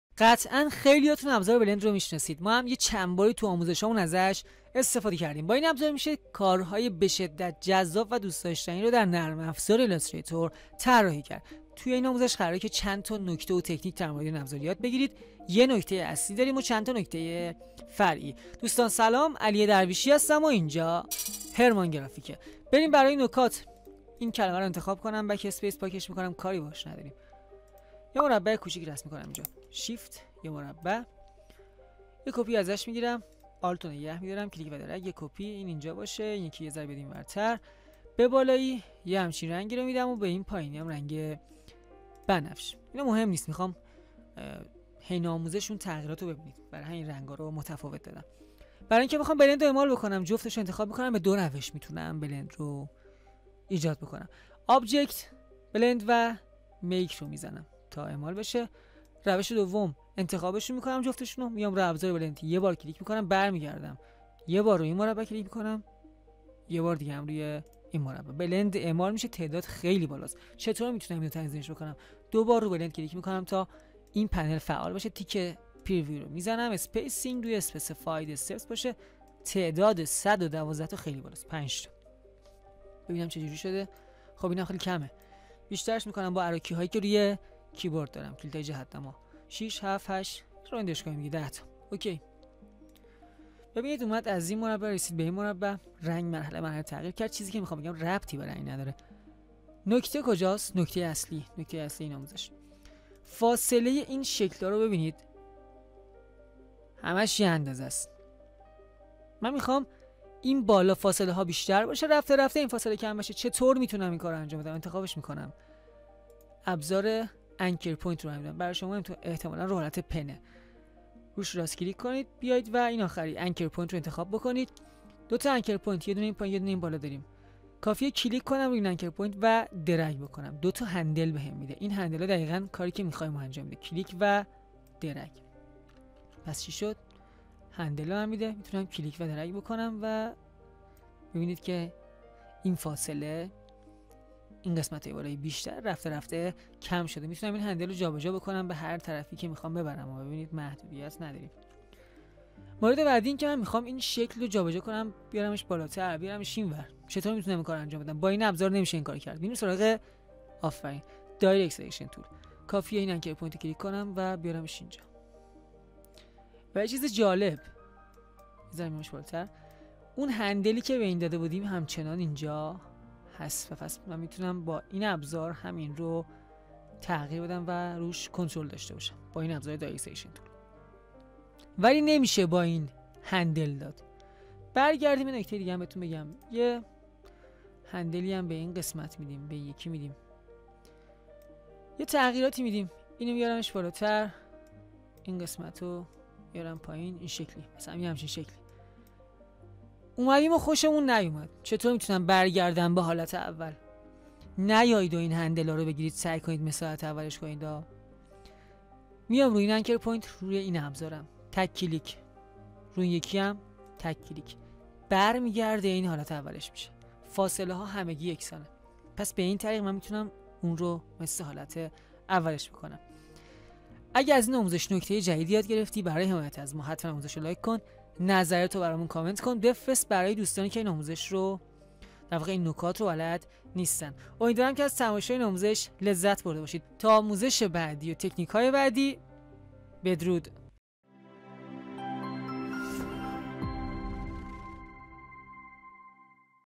آموزش ایلاستریتور:ابزارblend tool
دوستان سلام

امیدوارم روز خوبی رو پشت سر گذاشته باشید.
با ابزار blend در نرم افزار ایلاستریتور میشه طرح های زیادی ایجاد کرد و کاربردهای زیادی داره. در این آموزش قراره چندتا نکته درمورد این ابزار بگیم که کارکردن باهاش رو راحت تر میکنه و میتونید طرح های جذابی ایجاد کنید.

خیلی عشقید
پیج اینستاگرام: